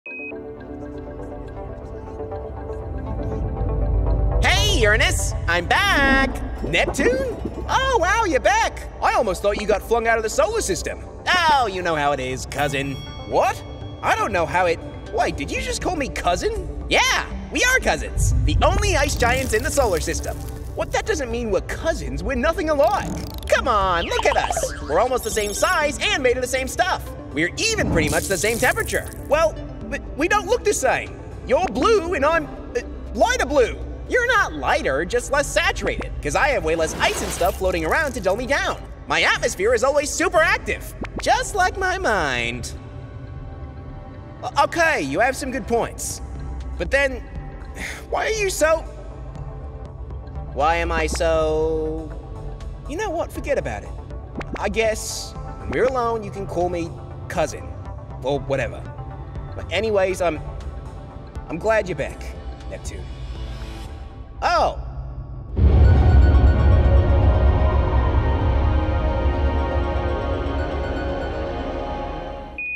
Uranus = Neptune?
Turns out Uranus and Neptune are quite similar.

To partner with us to VO SolarBalls in your own language, click:
shorturl.at/ikS19